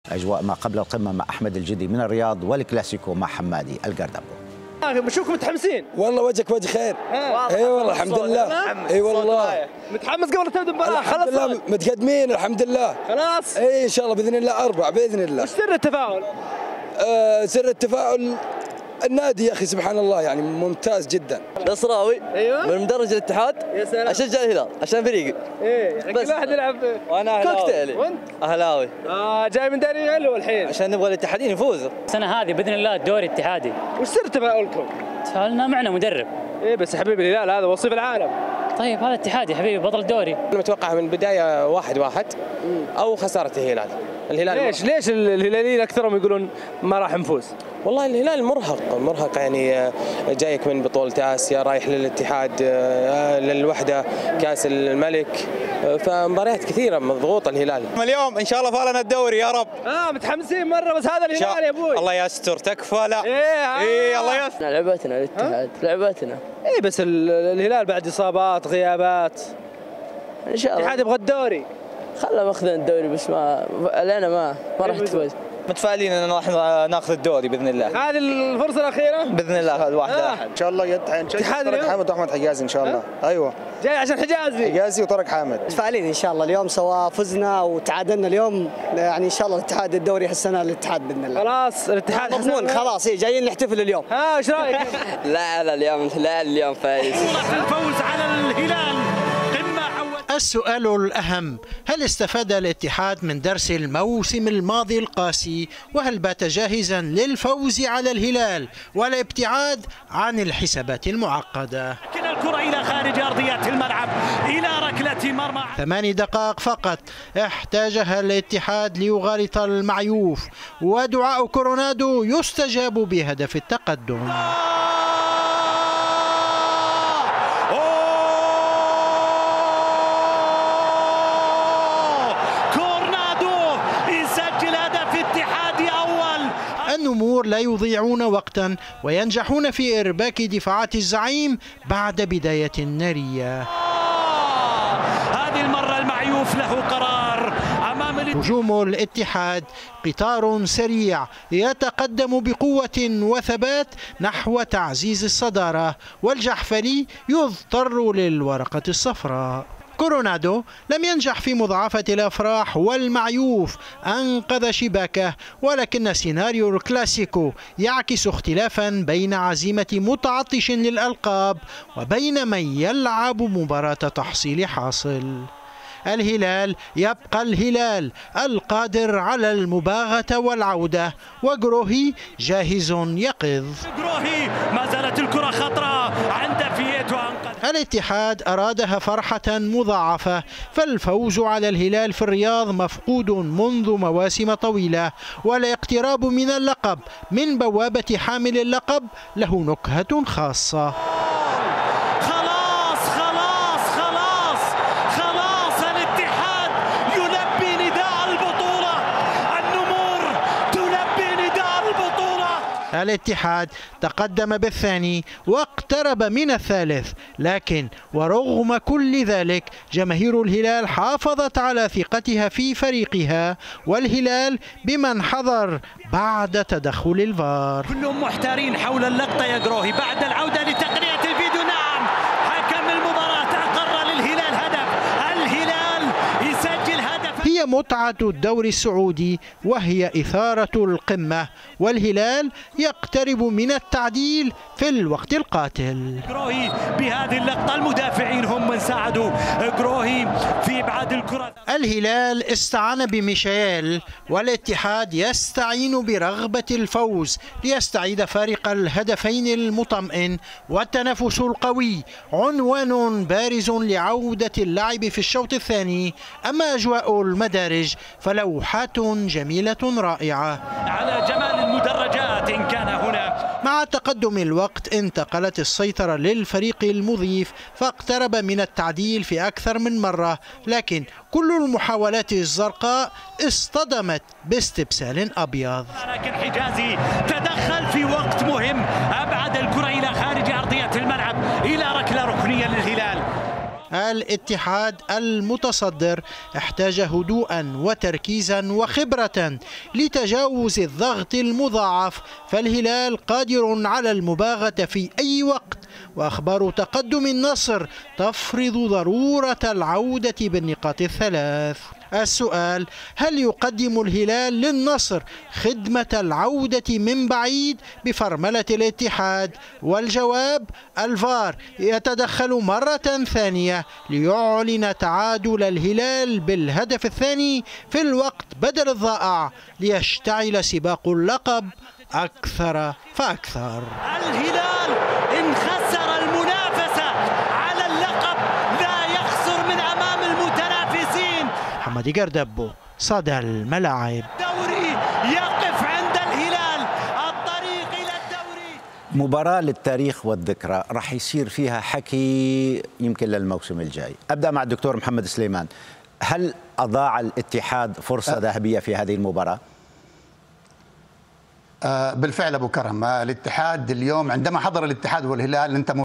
تعادل مثير بين الهلال والاتحاد في كلاسيكو ناري.. أجواء ما قبل المباراة وأبرز المحطات الفنية
تعادل مثير بين الهلال والاتحاد في كلاسيكو ناري.. أجواء ما قبل المباراة وأبرز المحطات الفنية في تقرير الصدى

مجانًا وبدون اشتراك على شاهد